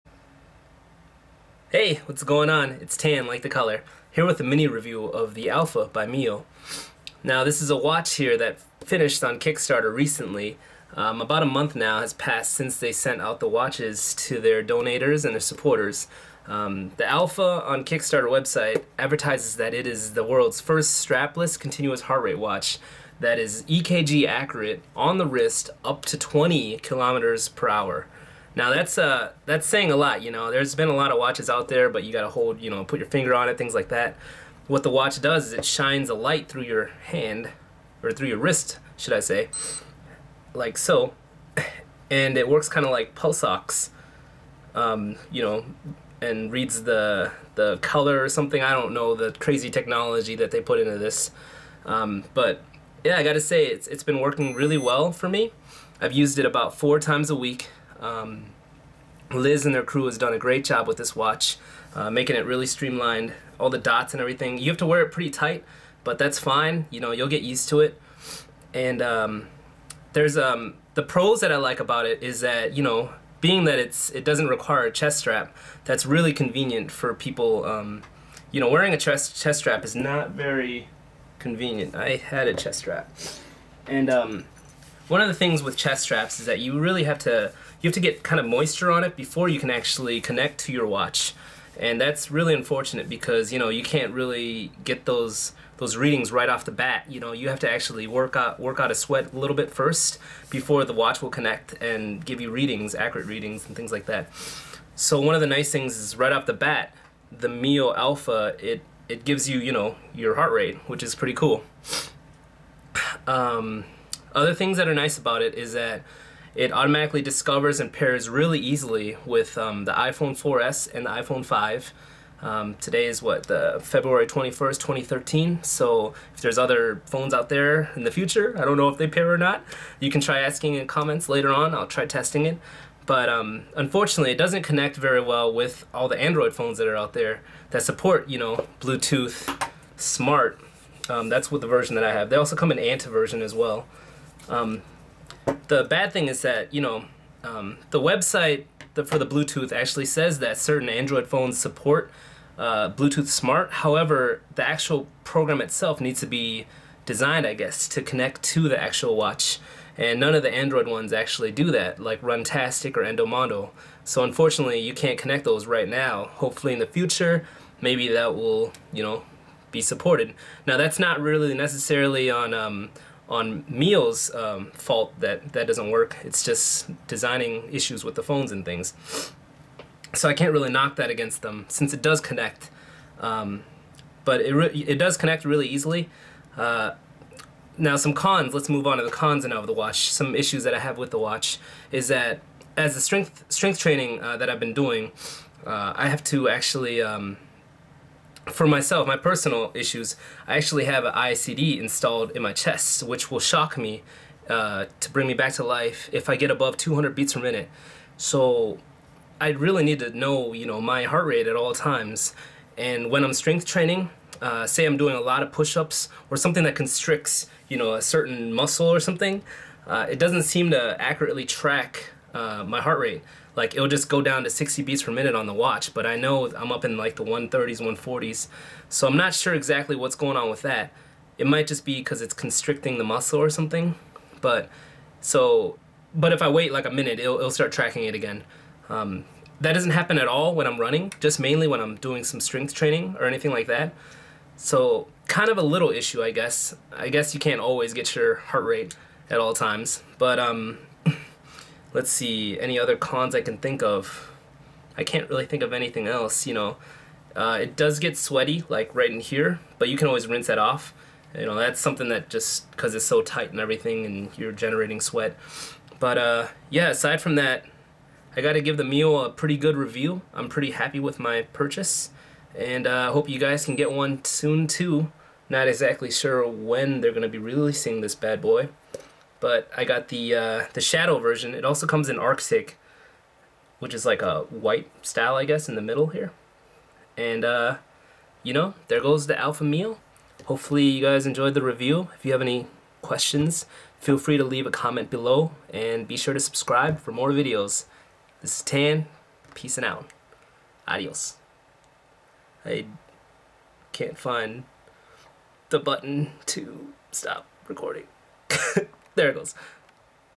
Review: Alpha by Mio (Strapless Heartrate Monitor)
Tan's miniature review of the world's first strapless heartrate monitor.